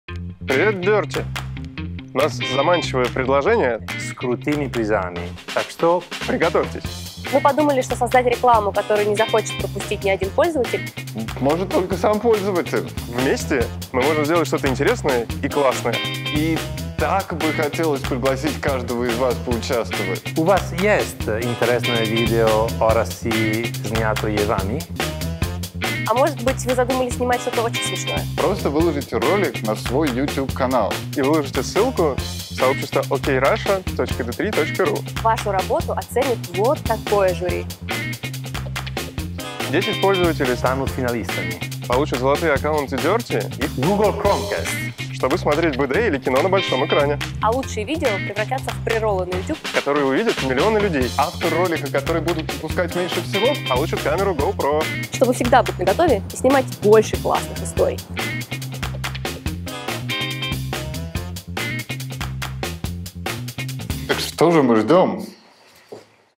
О'кей, Россия!
Поделись своим видео о России с 7 сентября по 3 ноября 2015 и создай крутую рекламу вместе с Google. Возможно, именно твой ролик увидит весь российский YouTube. Победителей ждёт слава и ценные призы.